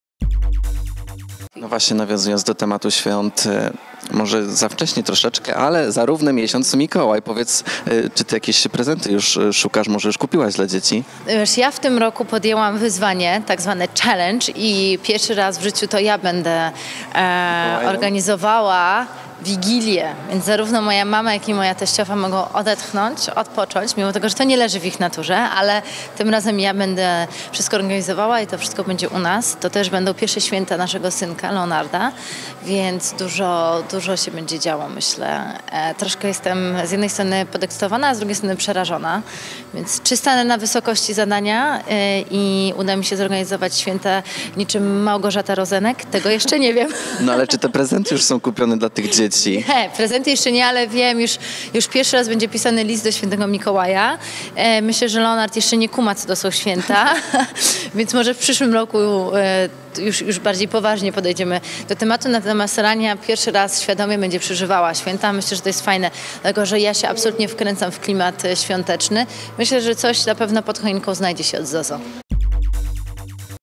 Zosia Ślotała jak Małgorzata Rozenek!? Uda się jej zorganizować święta jak ...
Sesja zdjęciowa ZoZo Design. Biżuteria modowa - 06.11.17 r.